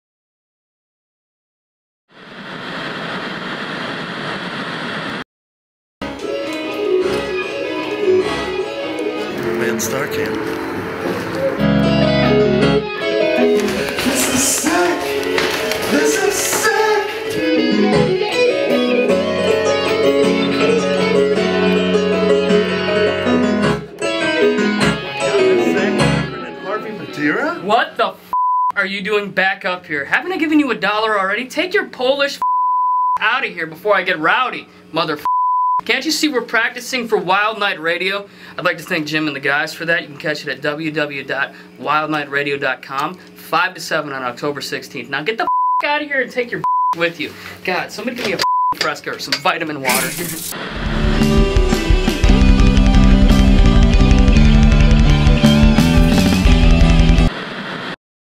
Star Cannon Radio Show
Check out the mysterious men of STAR CANNON as they shred your face to pieces on October 16th 5-7pm on wildniteradio.com